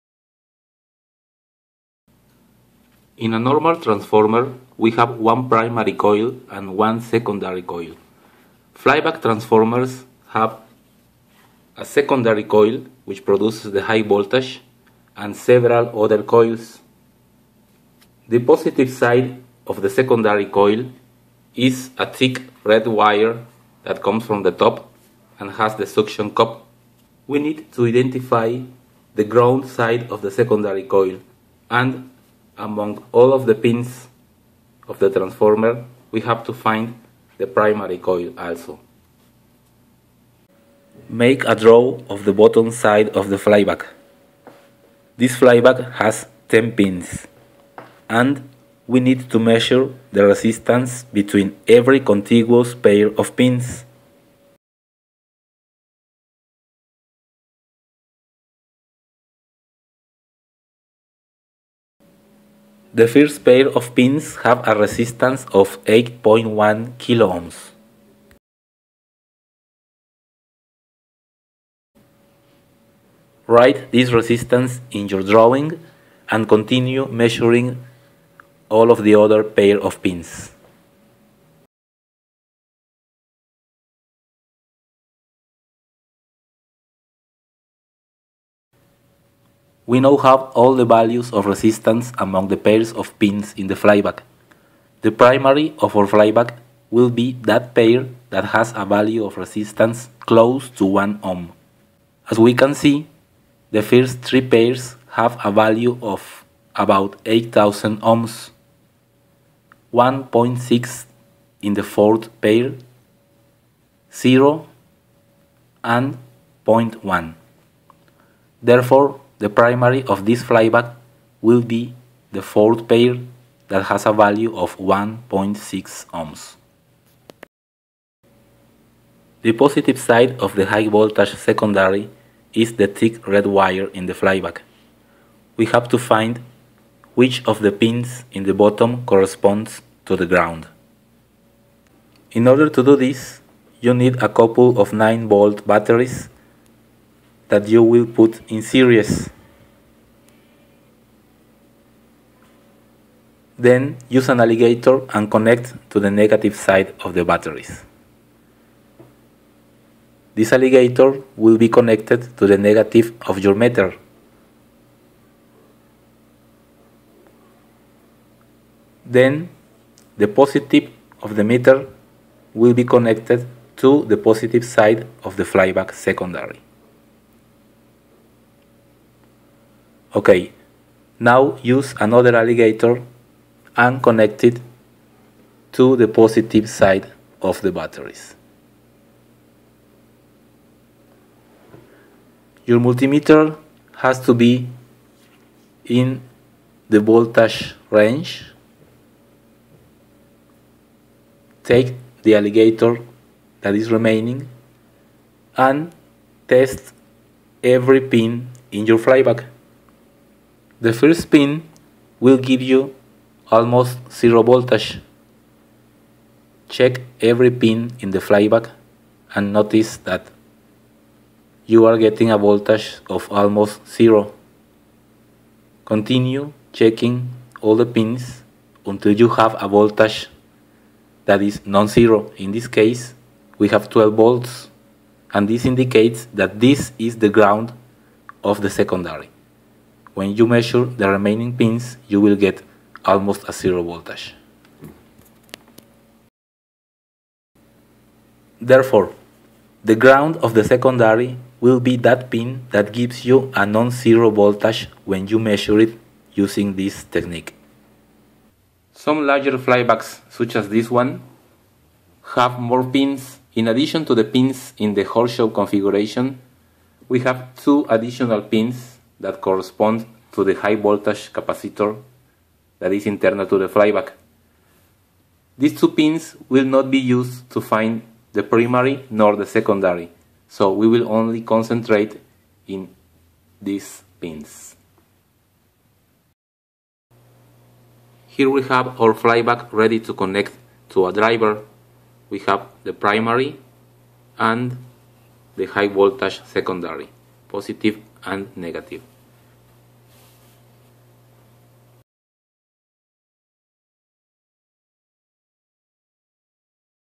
How to Identify the Primary and Secondary of a Flyback Transformer
This video will show you a simple way to id the pins of a flyback using a multimeter and two 9 volt batteries.

Please note that the best way is to look at the flyback's datasheet, but if you do not have the part number, or the datasheet is not available, this method can help (however not all flybacks can be tested with this procedure).